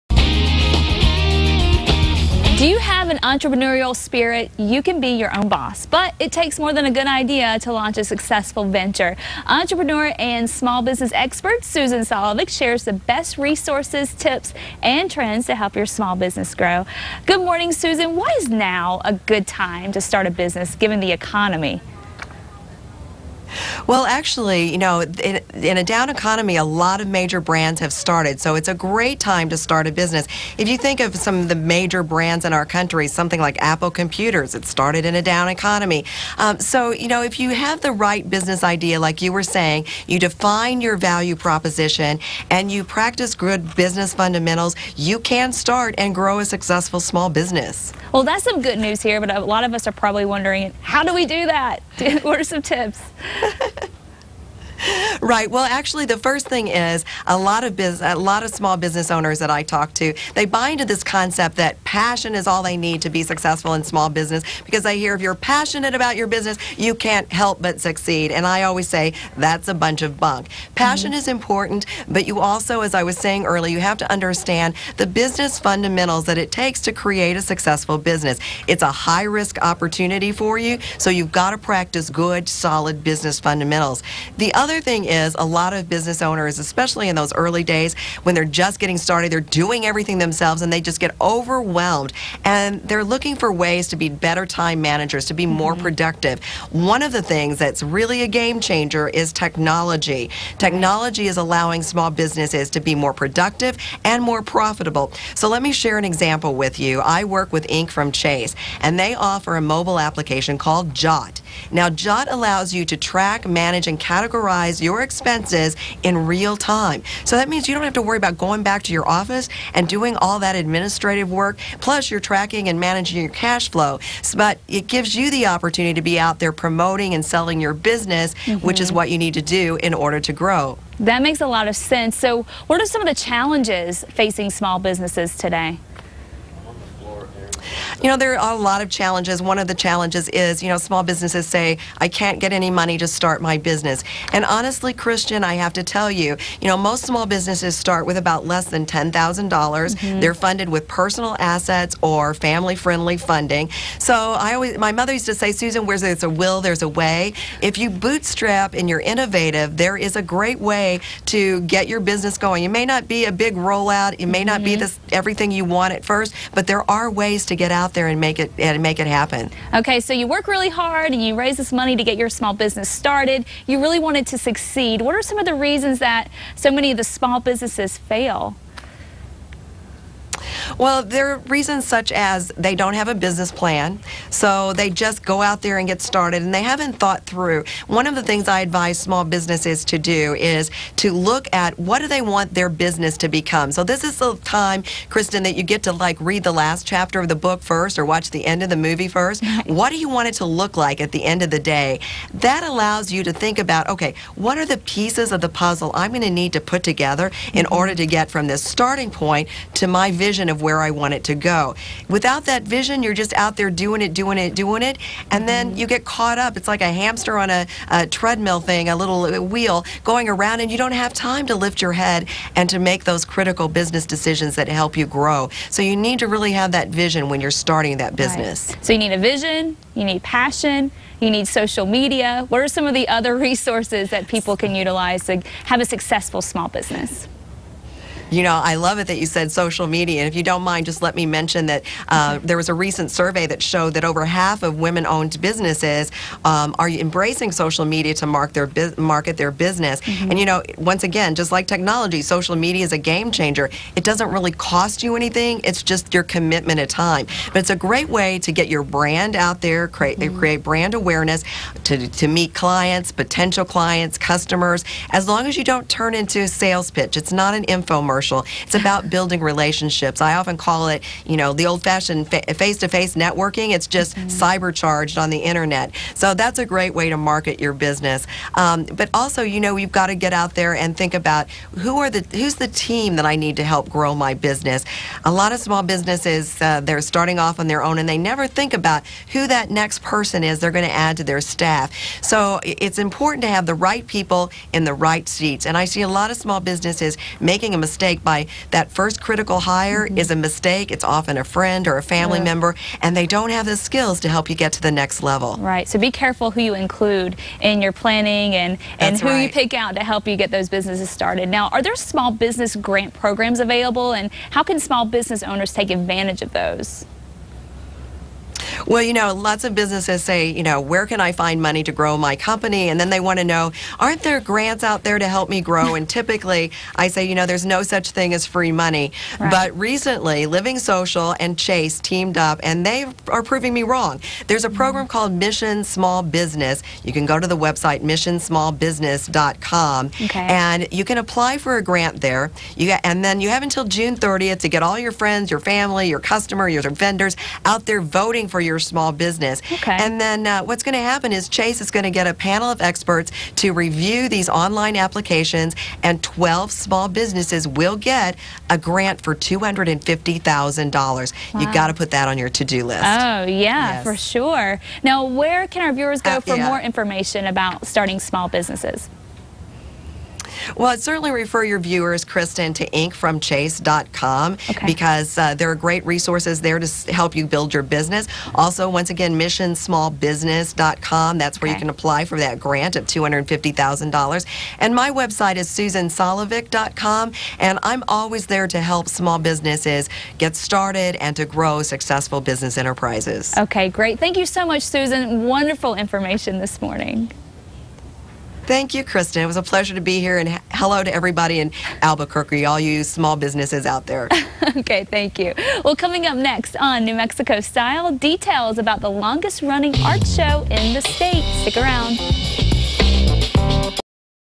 Building Small Business
Kristen talks to Entrepreneur and Small Business Expert Susan Solovic, who shares the best resources, tips, and trends to help your small business grow.